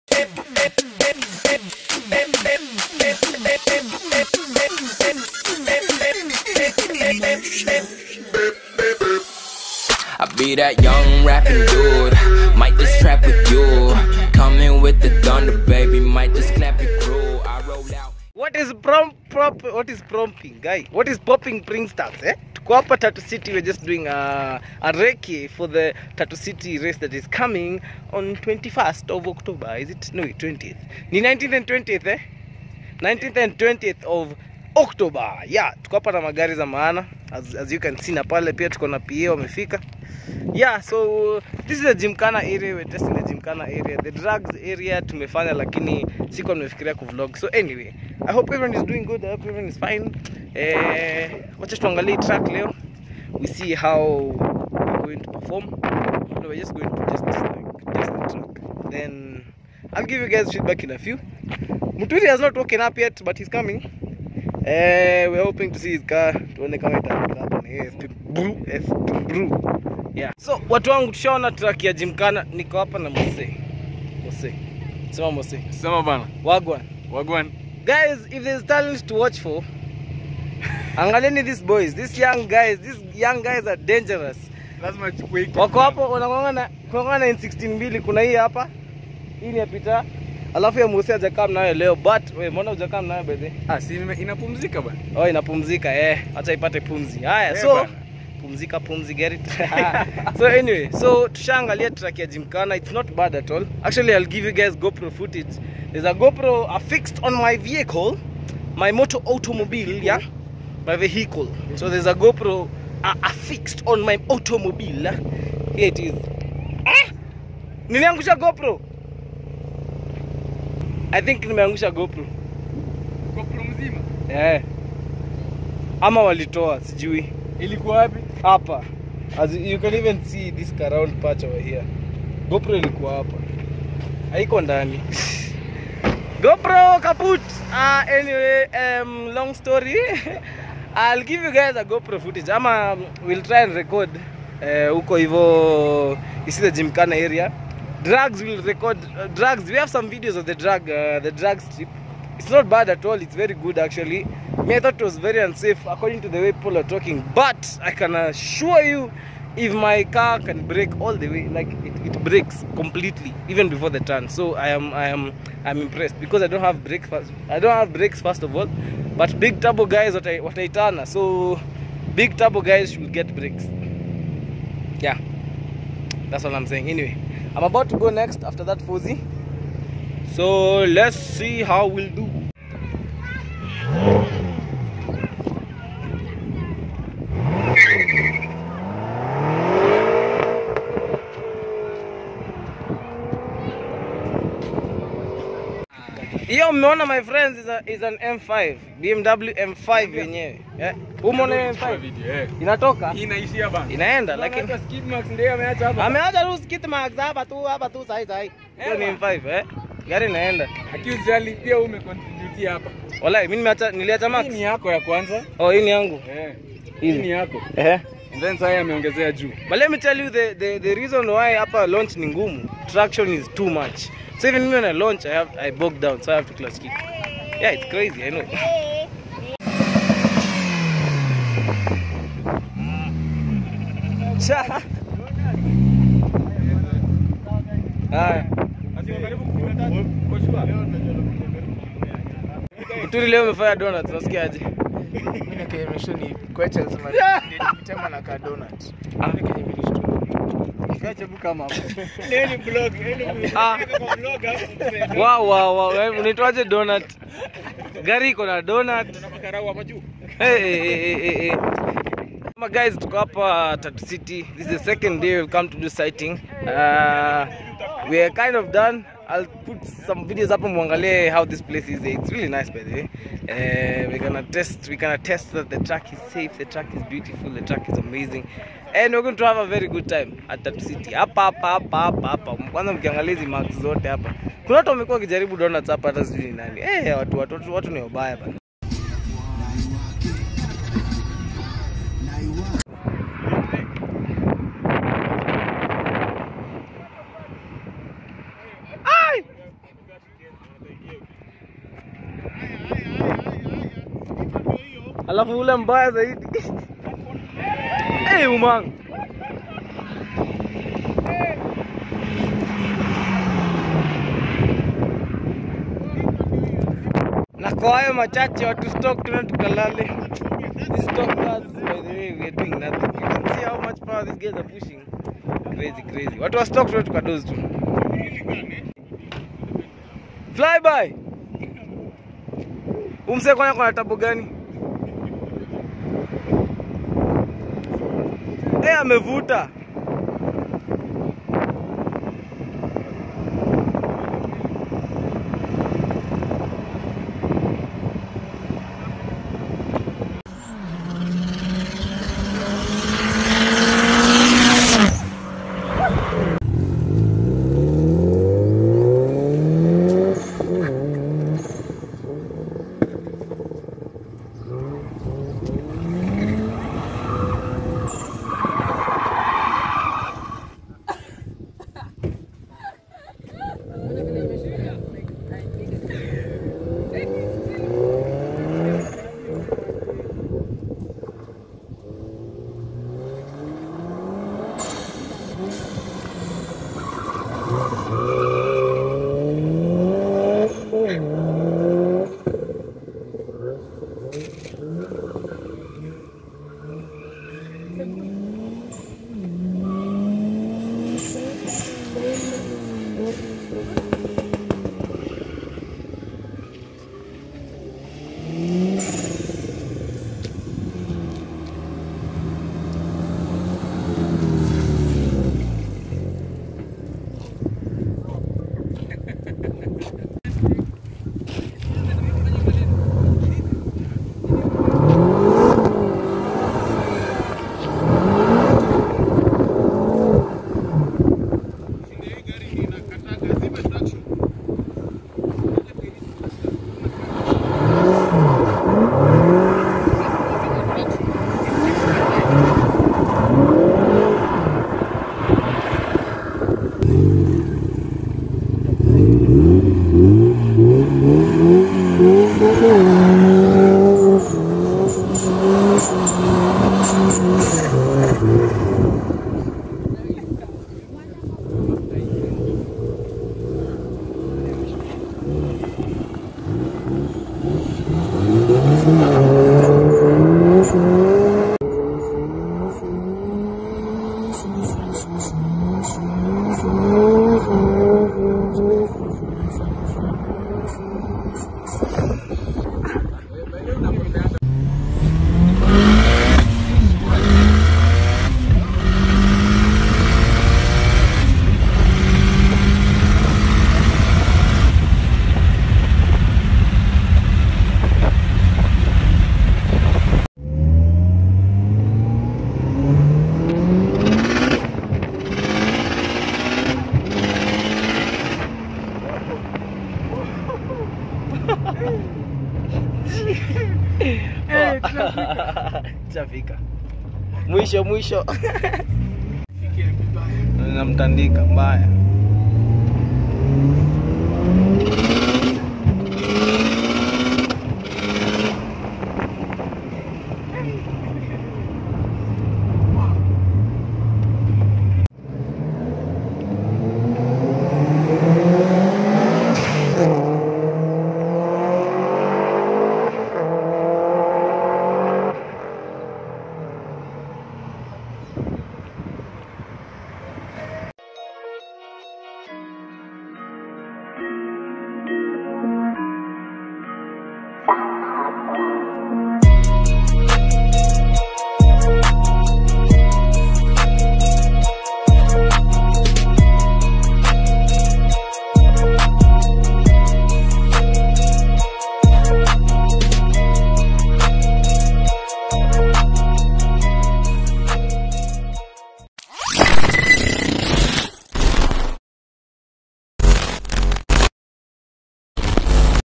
Racewars 2k19 Recce- Tatu City
This was the Reece done by a few of the racers at the amazing Venue in Tatu City, Gotta love it!!!